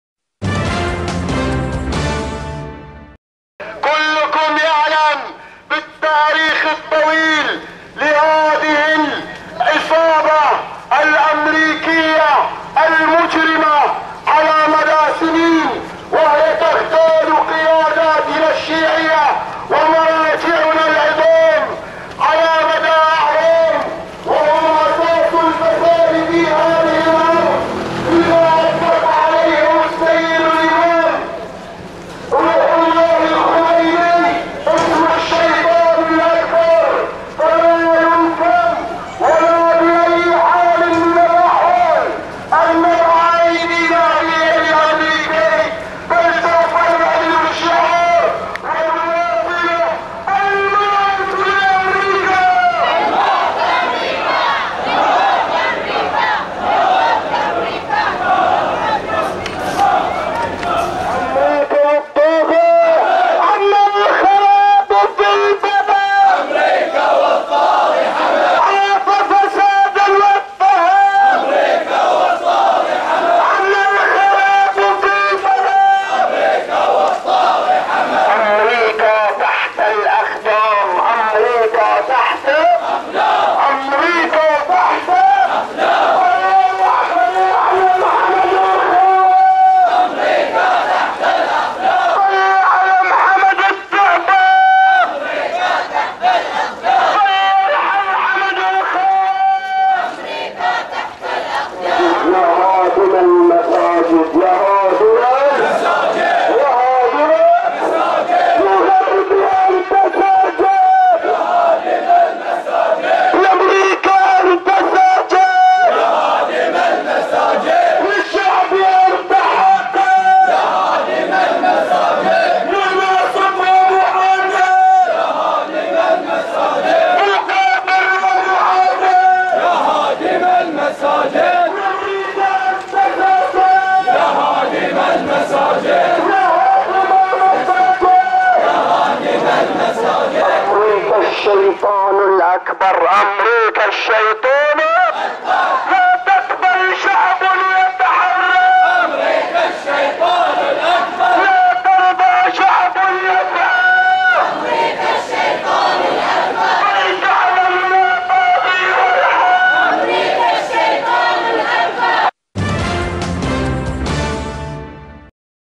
حشود غفيرة بمسيرة "أمريكا تحت أقدامنا" في سترة واديان 2014\4\25 Bahrain
قمعت قوات النظام الخليفي يوم أمس الجمعة (25 ابريل/نيسان 2014) مسيرة خرجت بمنطقة واديان في جزيرة سترة تحت شعار "أمريكا تحت اقدامنا" تنديدا بالتآمر الأمريكي على الشعب البحريني، وشارك في المسيرة حشود غفيرة حملوا أعلام ورايات متنوعة ورددوا شعارات مناهضة للنظام الخليفي. ولم تمهل قوات النظام وصول المسيرة السلمية لنهايتها حتى عمدت على قمع المشاركين بالغازات السامة ومسيلات الدموع.